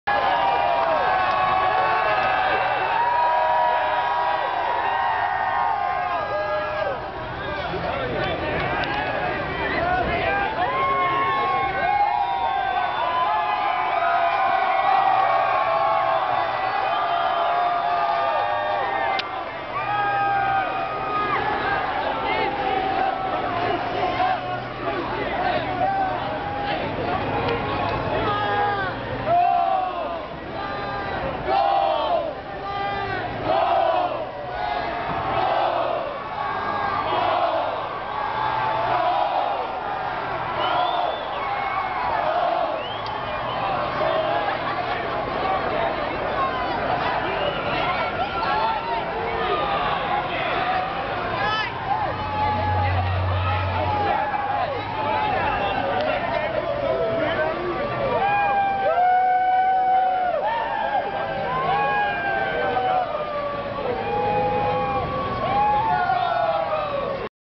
The Gates to Bright House Networks Open!
This is the scene for the main entrance gates to UCF's Bright House Networks Stadium as they opened to the public for the first time. UCF was defeated by #6 Texas on that day in what was an awesome game with an electric atmosphere.